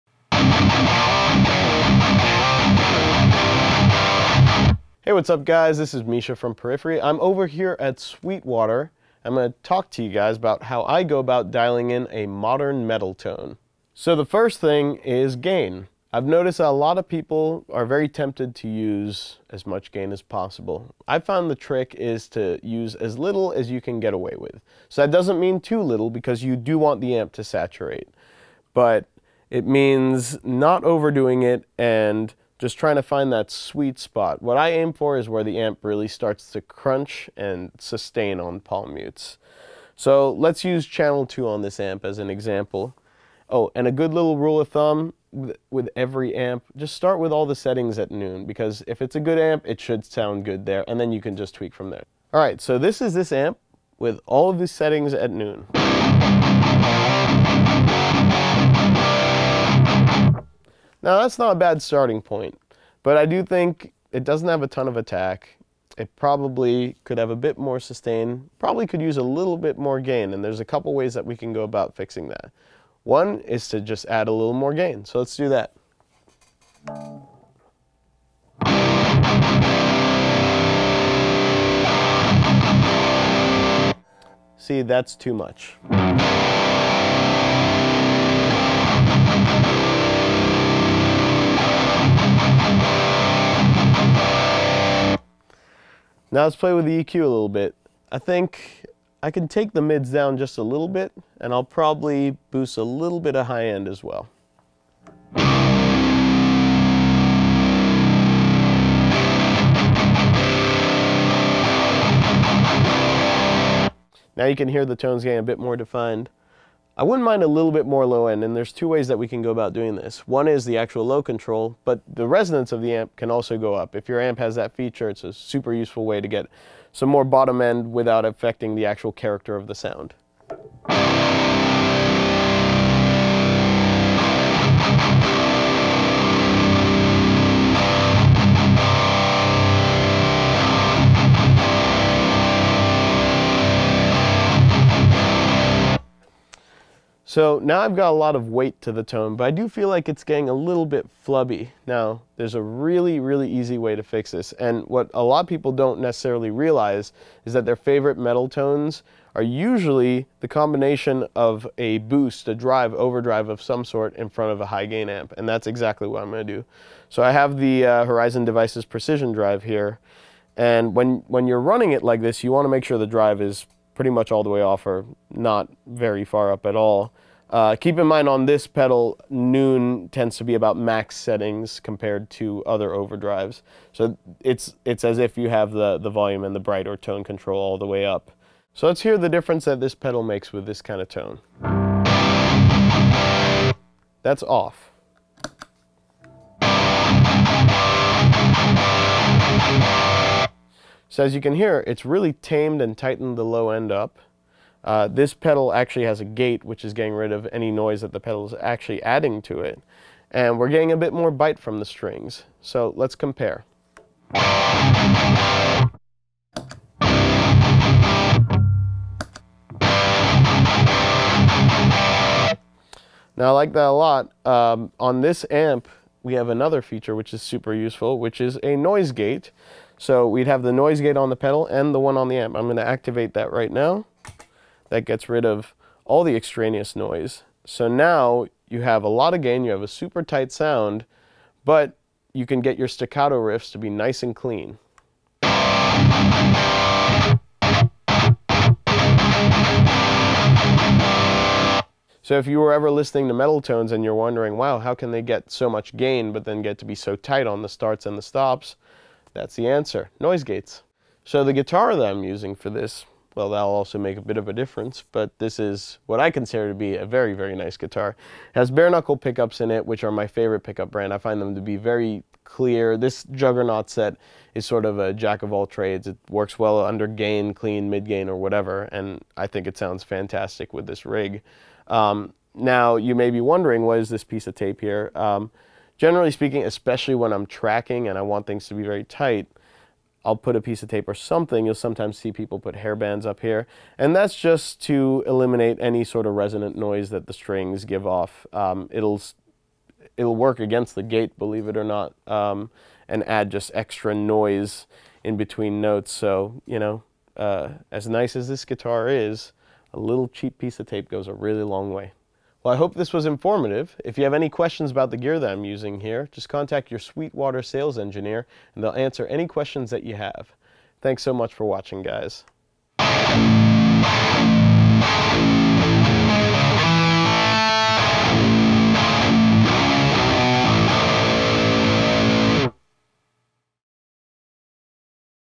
How to Dial In a Modern Metal Tone with Misha Mansoor
Modern metal maven Misha Mansoor from Periphery shows you how to dial in aggressive metal guitar tone. Misha is playing his Jackson USA signature guitar through the Peavey Invective.120 tube amp, using the Horizon Devices Precision Drive as a boost.

0:00 - Intro
0:15 - Gain
1:39 - EQ
2:33 - Precision Drive
3:48 - Noise Gates
4:24 - Guitar & Pickups
5:15 - Thanks For Watching!